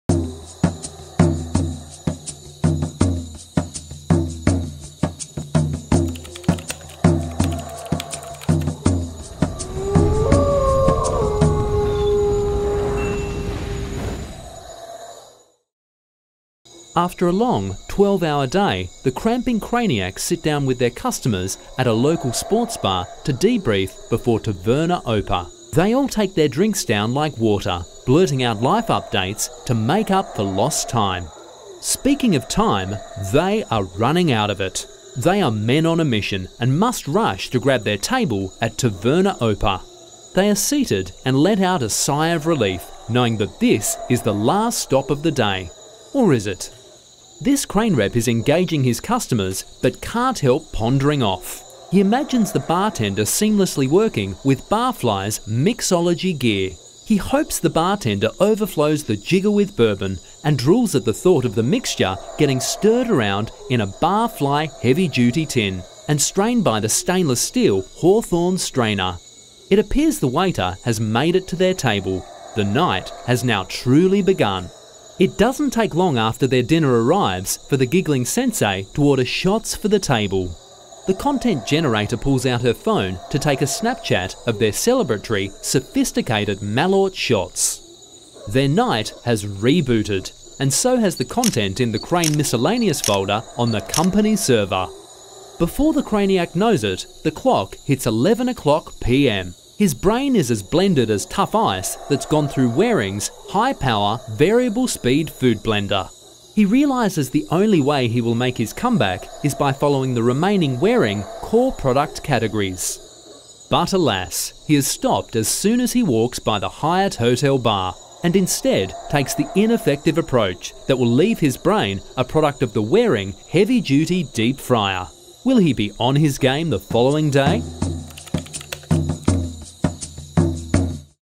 National Chrane-ographic: The NAFEM Series (Part Four)
A National Geographic inspired glimpse into what it would look like if Chrane went to The 2021 NAFEM Show. Tune in to part four of our five part series!